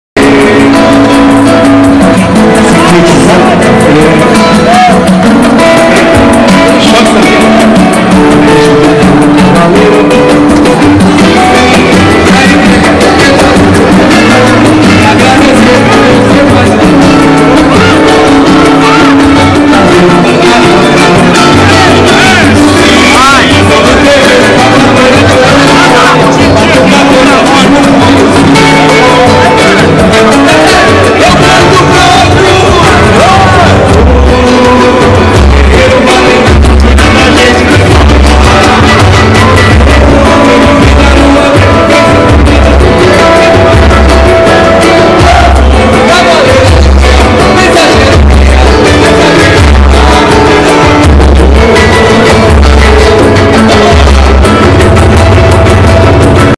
Roda de samba com Denilson, Marcos Assunção e Amaral os boleiros, boleragem
Samba dos amigos com Denilson, Amaral, Marcos Assunção, Ademir Fogaça, Pimpolho e amigos...